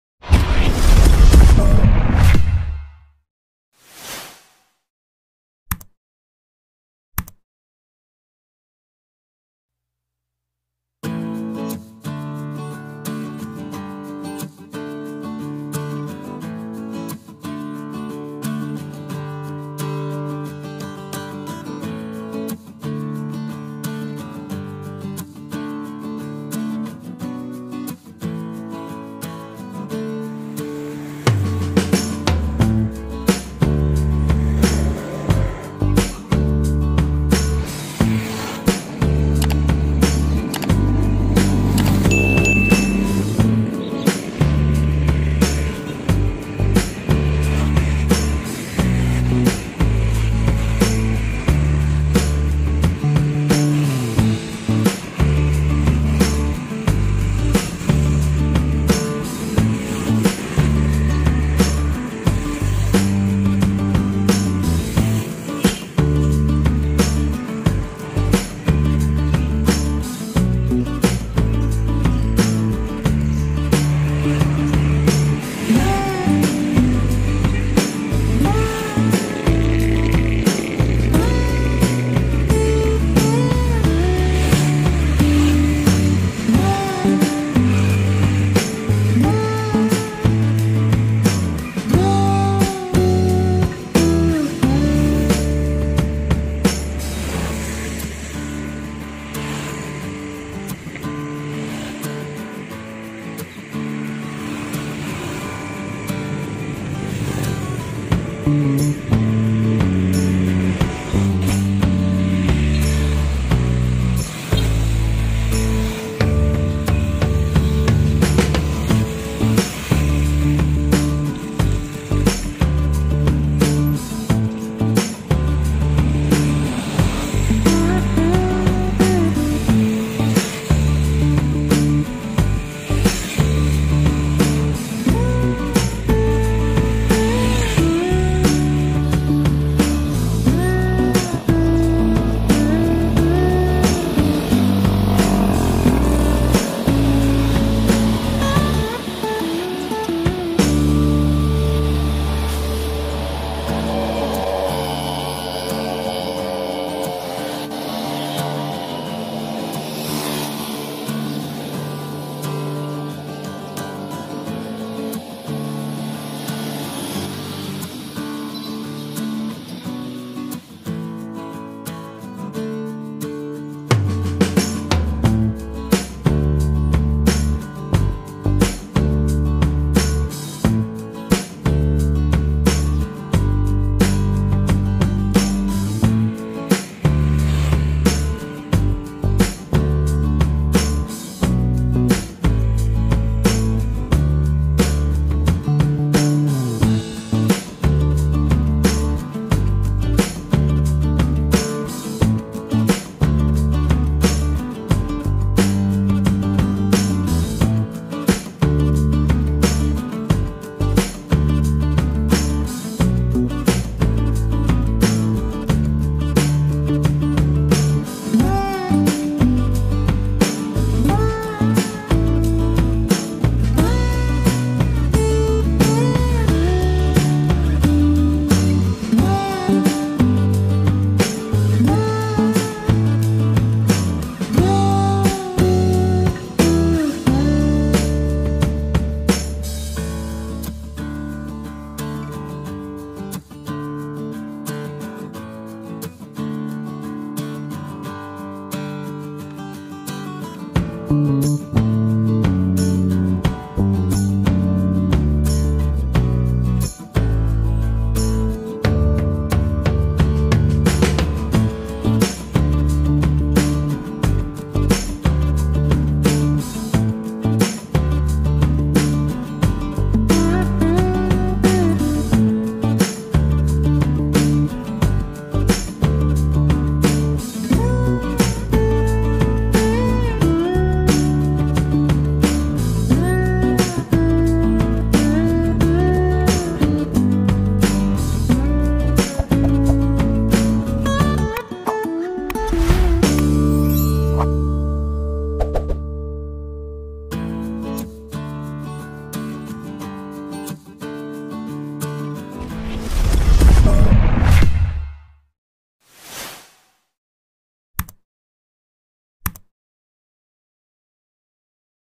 Tamed cows, lying down, roaming and grazing beside the road
Livestock, some cows roaming the side of the road, some lying down and some grazing